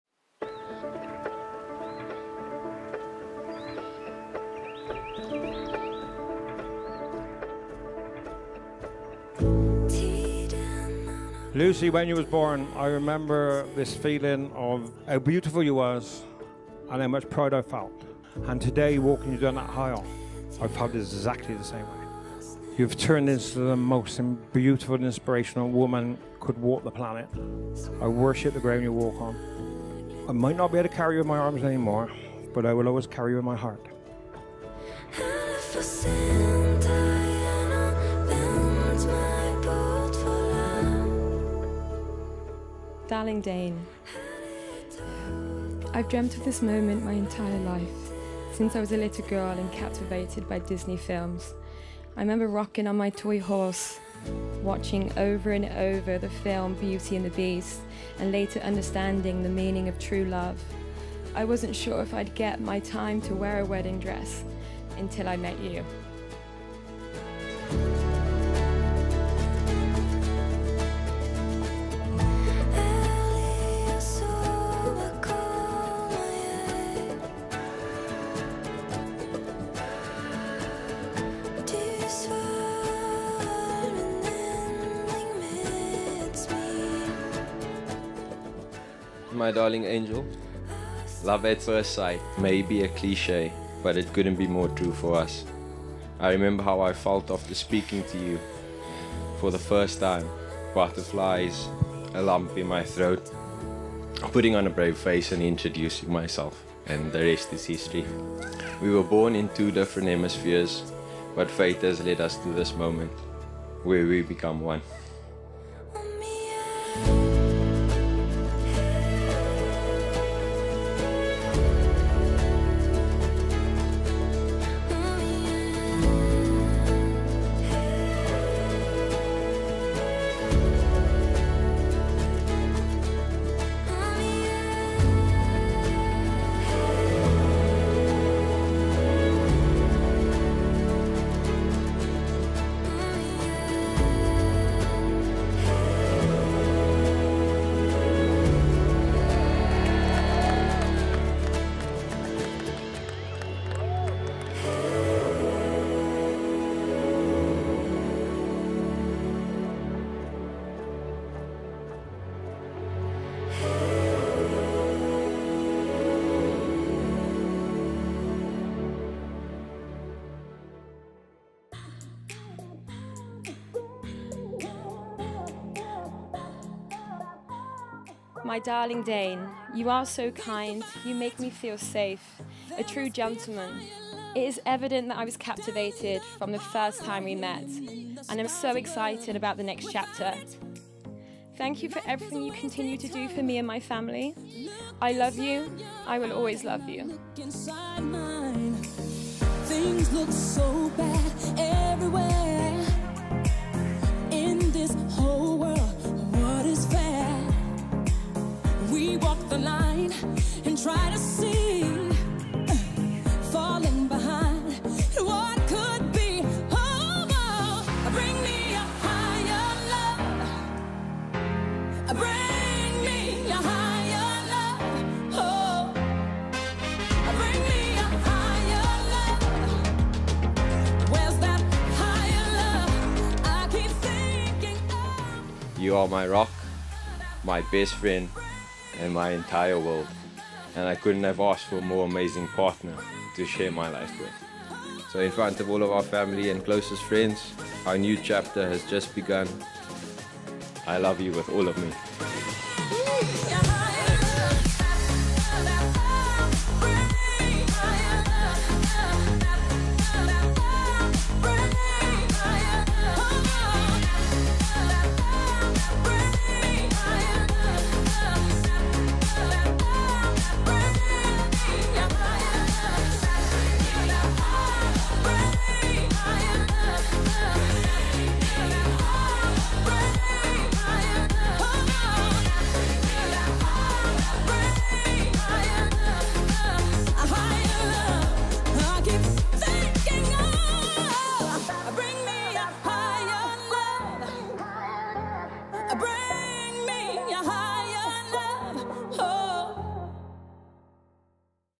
Dane & Lucy | Vrede en Lust Wedding Video
Lucy had her fairytale, childhood dream come true, marrying her soulmate Dane at the beautiful Vrede en Lust in Franshhoek, South Africa.